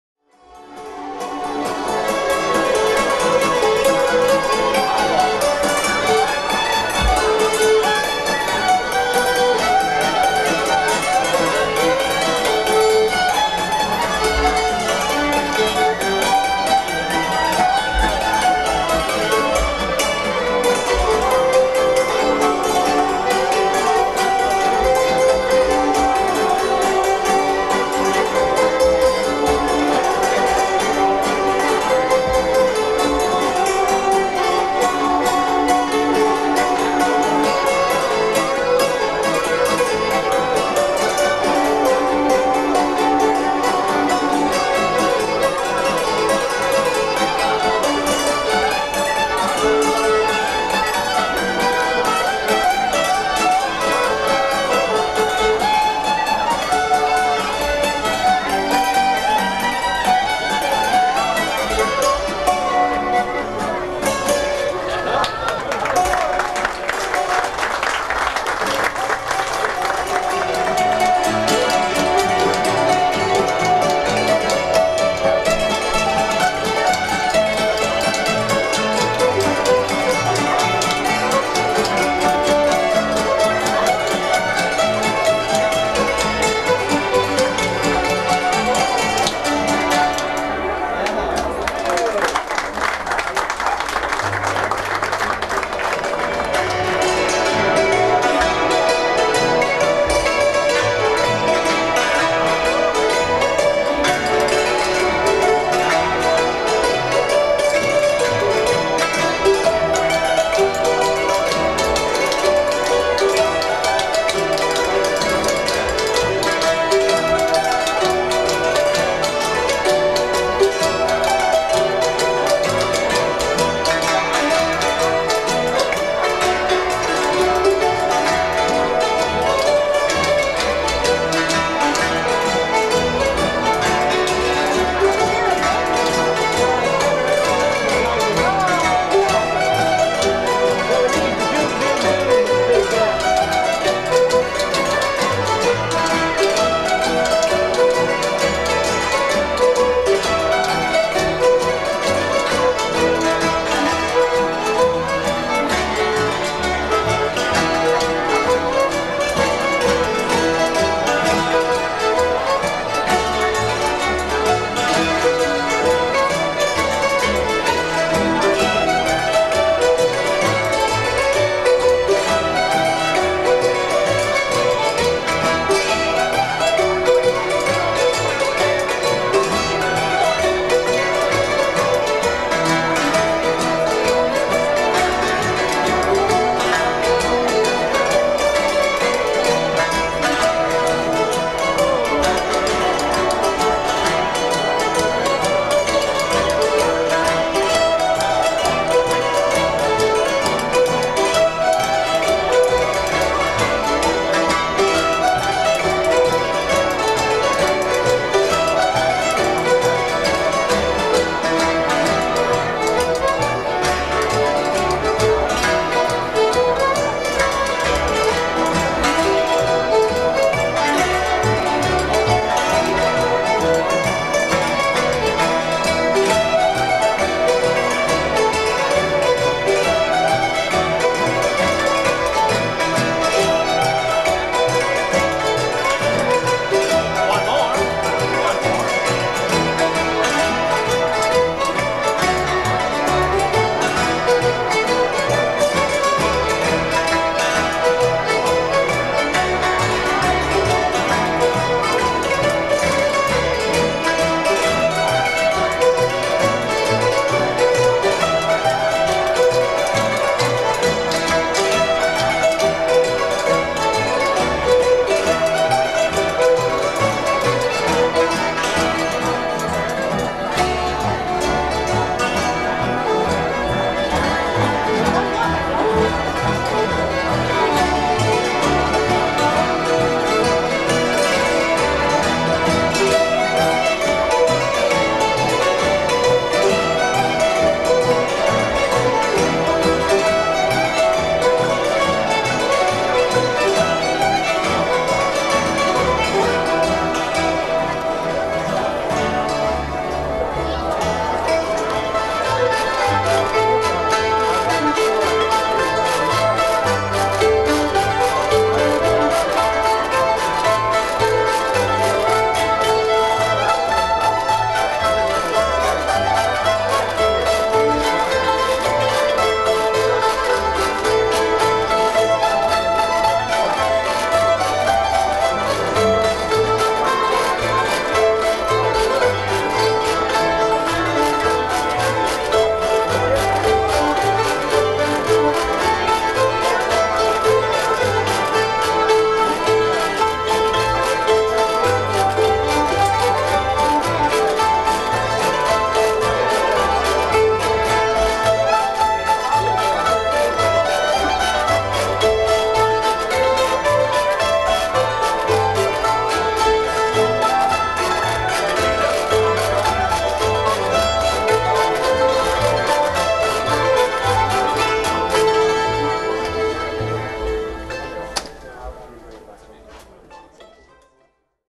Jam in vendors, Evart 1982
A typical evening jam back when the vendor pavilion was available for jamming.  From the 1982 ODPC FunFest in Evart Michigan.